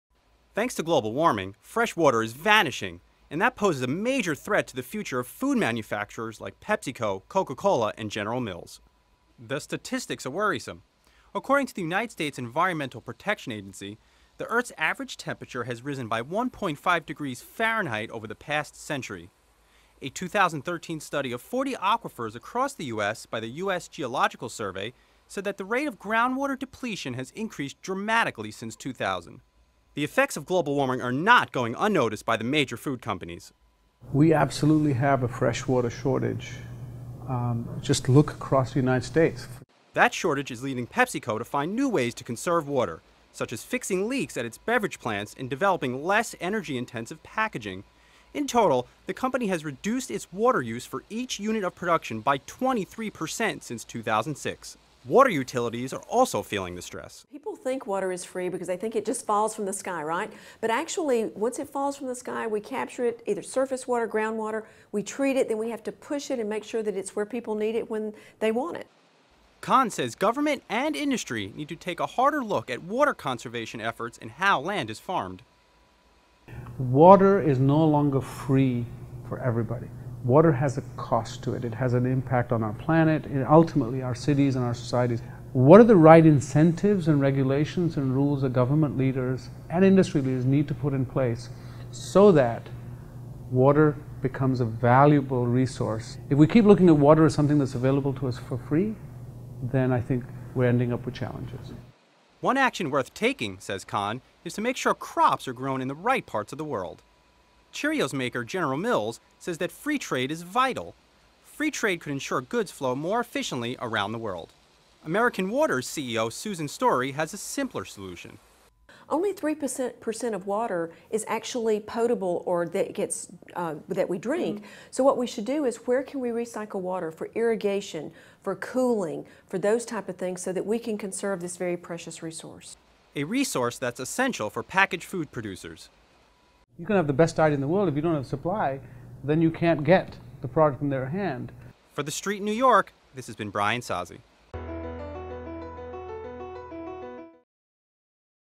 Fresh Water Is Vanishing, and That’s a Major Problem For Food Manufacturers
Thanks to global warming, fresh water is vanishing. And that poses a major threat to the future of food manufacturers like PepsiCo (PEP), Coca Cola (KO) and General Mills (GIS). The statistics surrounding global warming are getting worrisome. According to the United States Environmental Protection Agency, the Earth's average temperature has risen by 1.5 degrees Fahrenheit over the past century. A 2013 study of 40 aquifers across the U.S. by the U.S. Geological Survey said that the rate of groundwater depletion has increased dramatically since 2000. Subsequently, the effects of global warming are not going unnoticed by the food companies and water utilities. ‘We absolutely have a fresh water shortage, just look across the United States, the droughts in California, and everywhere else,’ said PepsiCo vice chairman and Chief Scientific Officer Dr. Mehmood Khan in an interview with TheStreet. That shortage has caused PepsiCo to find new ways to conserve water, such as fixing leaks at its beverage plants and developing less energy intensive packaging. In total, the company has reduced its water use per unit of production by 23% since 2006. TheStreet’s Brian Sozzi reports.